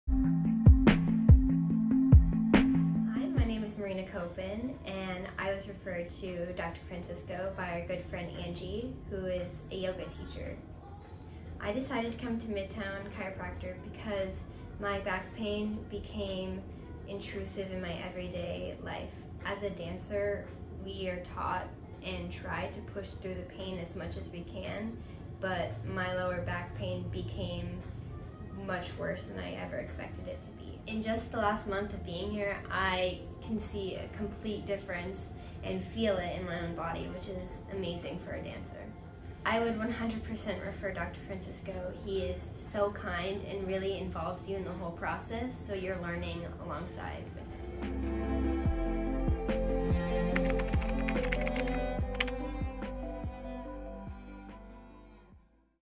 Midtown Clinic of Chiropractic- Testimonial Tuesday
Click play and listen to Marinas amazing experience here at Midtown!